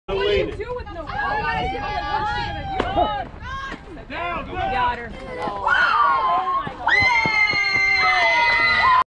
Throw down to 2nd
Solid throw to second. 1st tourney Fall 2022 with travel ball IYG Black Widows